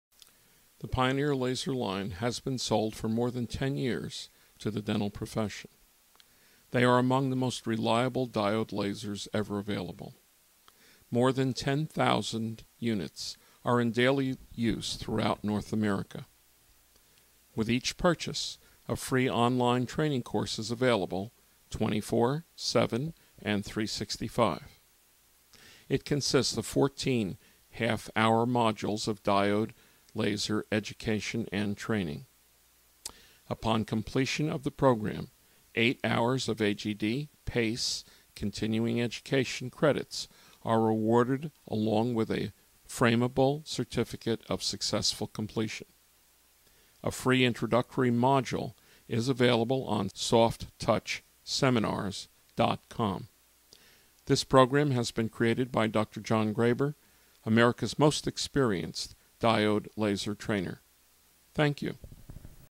Pioneer Lasers Overview
Pioneer Lasers are easy to use, assist dental practitioners in improving patients oral health and have a long history of reliability.